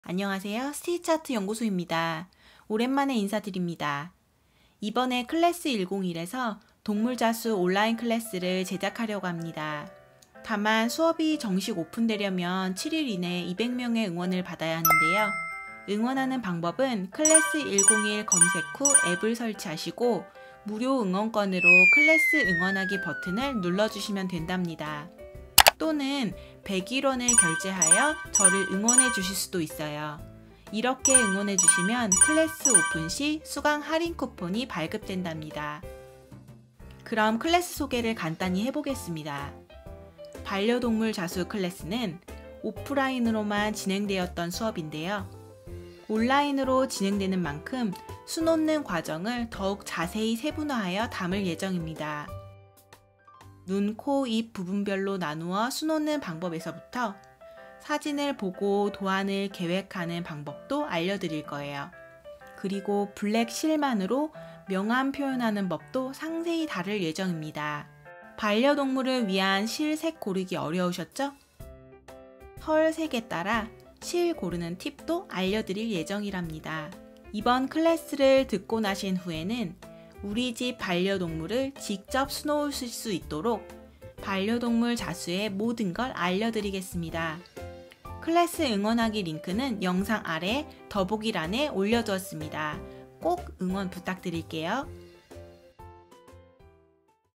[클래스101수요조사] 동물자수클래스 응원부탁드립니다.
클래스 응원하기 링크

안녕하세요. 클래스101에서 동물자수클래스를 기획하였습니다.
유튜브 특성상 자세히 담지 못하는 내용을
온라인클래스로 기획하였답니다.

정식 오픈된다면 거리, 시간상 배우러 오지 못하셨던 분들에게
기쁜소식이 될것 같습니다.
응원 많이많이 해주세요~
곧 정식 오픈되었다는 기쁜소식을 전해드릴게요.